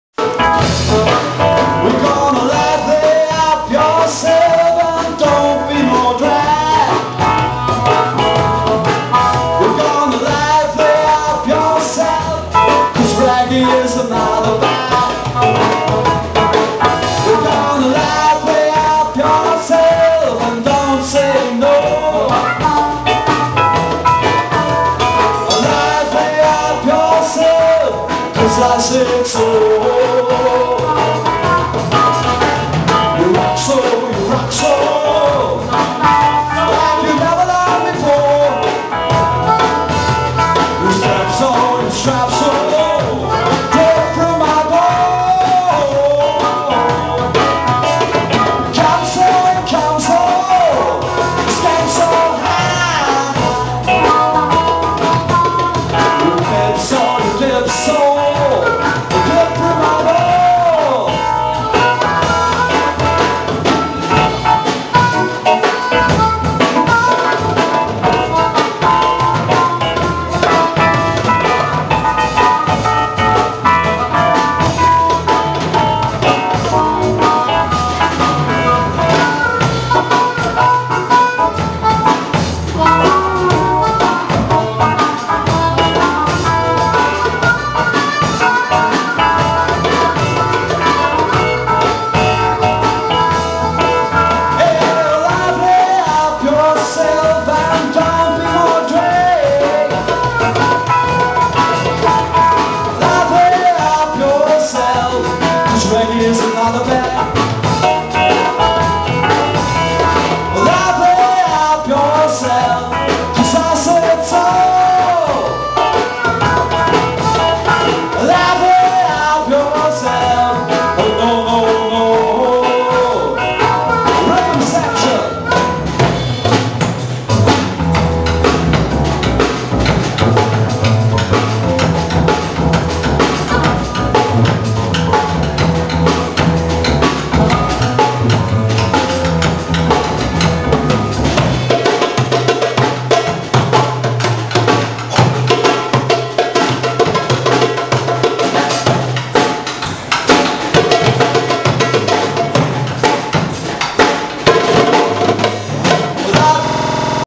8 8 taipei blues
海豚上台跟klaus等人一起玩樂,沒有套練,即時搭配...還好...海豚...歌曲都熟外加...反應快...哈哈哈